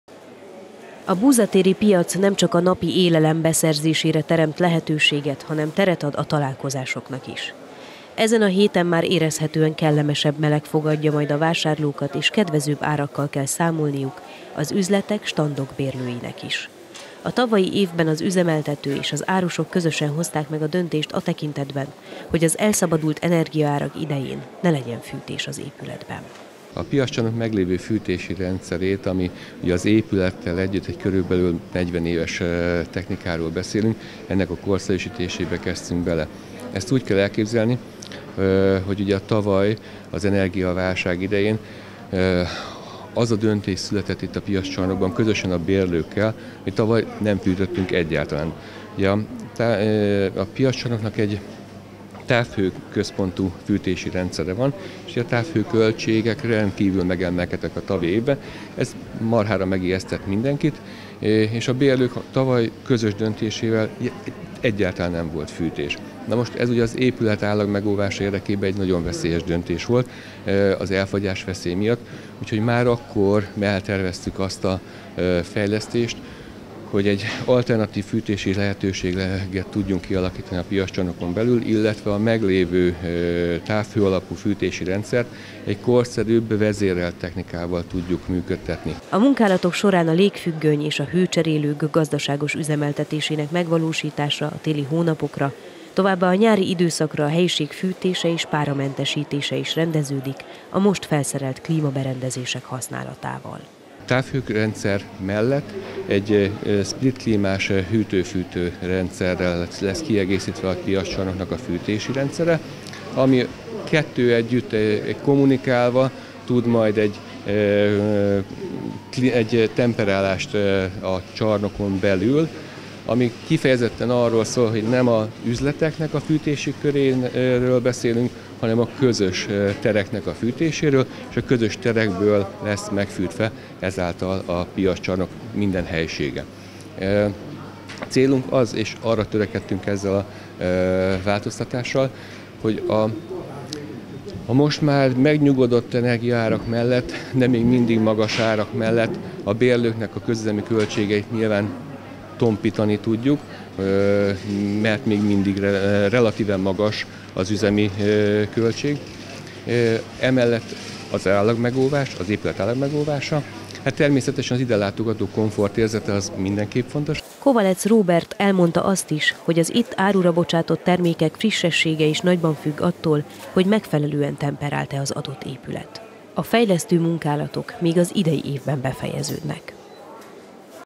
Komfortosabb piaci körülmények
Komfortosabb körülmények várják a nyíregyházi, Búza téri Piaccsarnok vásárlóit a jövőben.

A működtető NYÍRVV korszerűsítette az épület energetikai rendszerét. Felújították mind a fűtési, mind a hűtési hálózatot. A beruházás 8,5 millió forintba került.